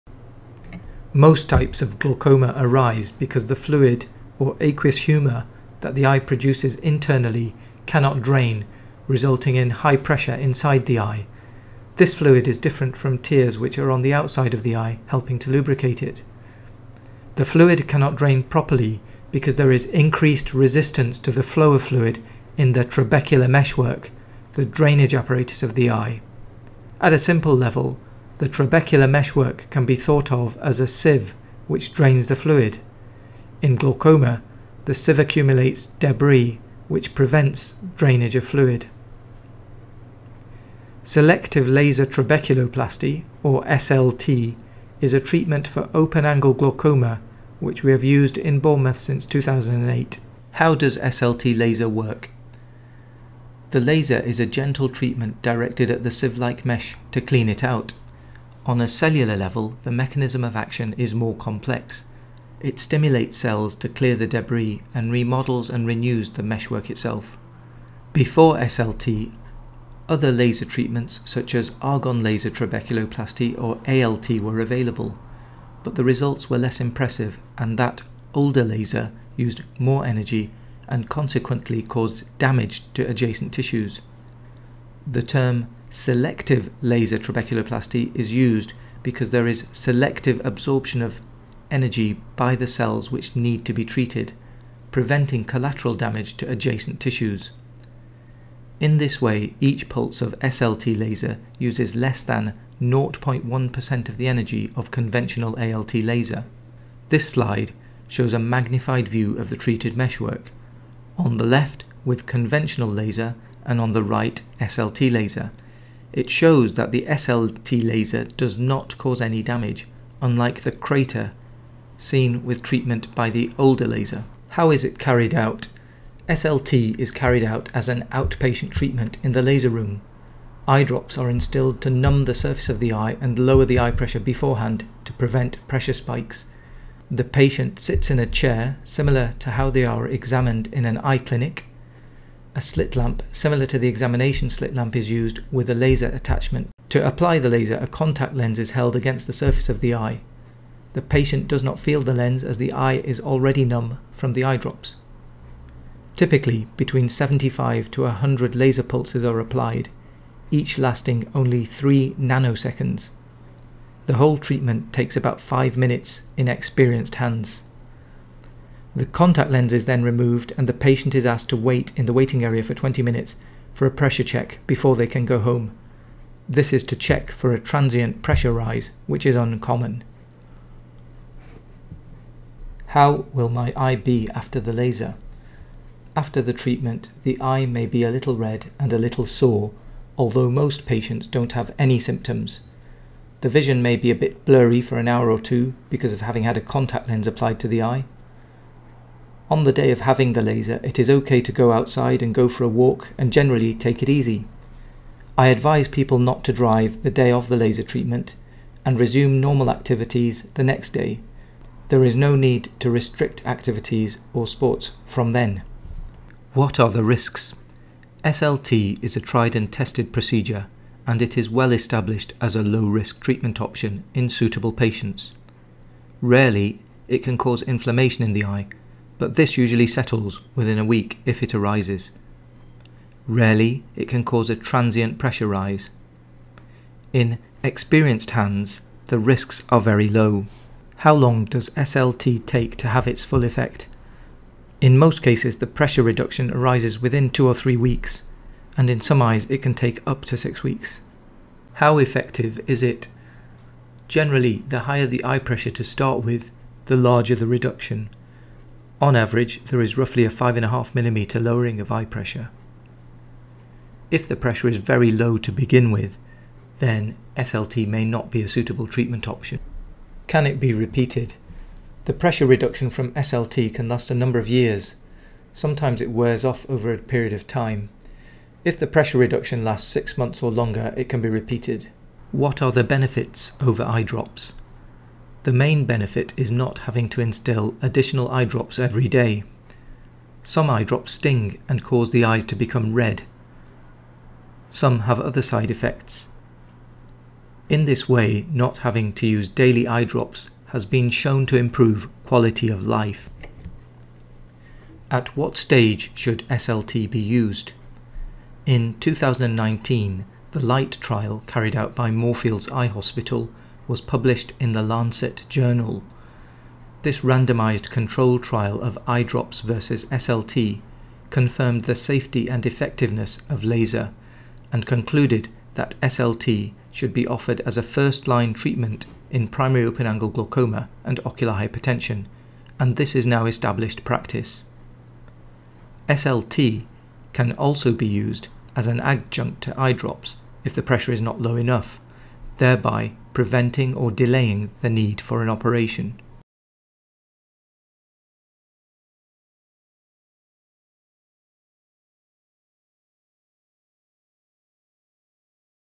Selective laser trabeculoplasty SLT, glaucoma treatment
What is selective laser trabeculoplasty or SLT ? How dos it work and how is it carried out ? The video discusses all about this new glaucoma treatment, its effectiveness and the science and research behind its use. It looks at the findings of Moorfields Eye Hospital LIGHT Trial and when SLT should be used.
you tube channel sightsurgeon
for more information about cataract and glaucoma